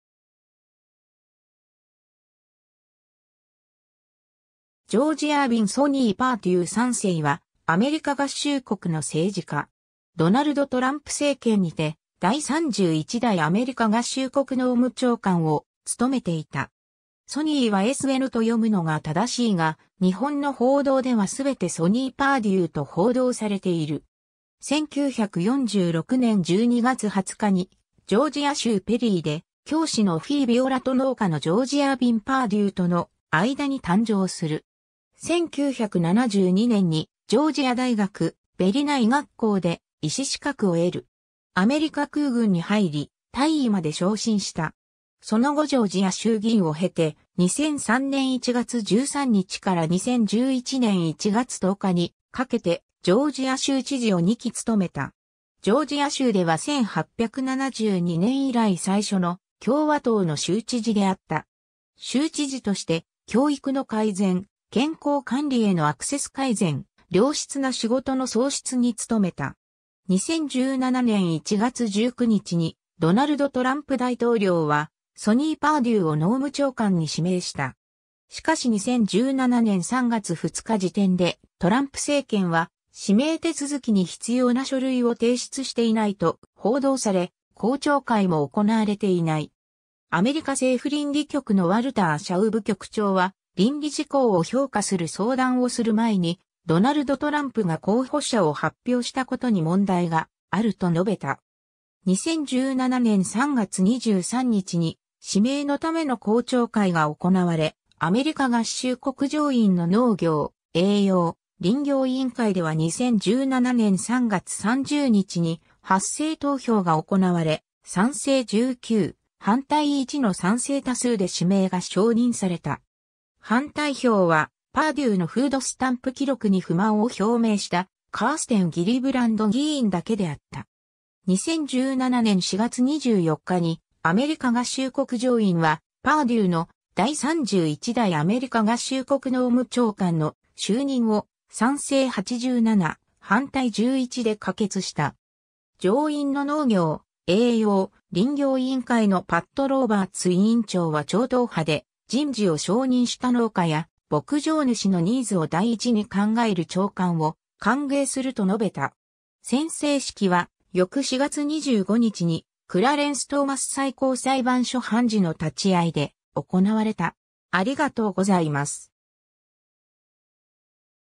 ソニー・パーデュー
ジョージ・アービン・"ソニー"・パーデュー3世（英語：George Ervin "Sonny" Perdue III、1946年12月20日 - ）は、アメリカ合衆国の政治家。
ドナルド・トランプ政権にて第31代アメリカ合衆国農務長官を務めていた。
 Sonnyはsʌnɪ（サニ）と読むのが正しいが、日本の報道では全てソニー・パーデューと報道されている。
 1946年12月20日にジョージア州ペリーで、教師のオフィー・ヴィオラと農家のジョージ・アービン・パーデューとの間に誕生する。
 1972年にジョージア大学ヴェリナ医学校で医師資格を得る。
 アメリカ空軍に入り、大尉まで昇進した。
その後ジョージア州議員を経て、2003年1月13日から2011年1月10日にかけてジョージア州知事を2期務めた。
ジョージア州では1872年以来最初の共和党の州知事であった。
州知事として教育の改善・健康管理へのアクセス改善・良質な仕事の創出に努めた。
 2017年1月19日にドナルド・トランプ大統領はソニー・パーデューを農務長官に指名した。
しかし2017年3月2日時点でトランプ政権は指名手続きに必要な書類を提出していないと報道され、公聴会も行われていない。
アメリカ政府倫理局のワルター・シャウブ局長は、倫理事項を評価する相談をする前にドナルド・トランプが候補者を発表したことに問題があると述べた。
 2017年3月23日に指名のための公聴会が行われ、アメリカ合衆国上院の農業・栄養・林業委員会では2017年3月30日に発声投票が行われ、賛成19・反対1の賛成多数で指名が承認された。
反対票はパーデューのフードスタンプ記録に不満を表明したカーステン・ギリブランド議員（ニューヨーク州選出・民主党）だけであった。
 2017年4月24日にアメリカ合衆国上院はパーデューの第31代アメリカ合衆国農務長官の就任を賛成87・反対11で可決した。
上院の農業・栄養・林業委員会のパット・ローバーツ委員長は超党派で人事を承認した農家や牧場主のニーズを第一に考える長官を歓迎すると述べた。
宣誓式は翌4月25日にクラレンス・トーマス最高裁判所判事の立ち合いで行われた。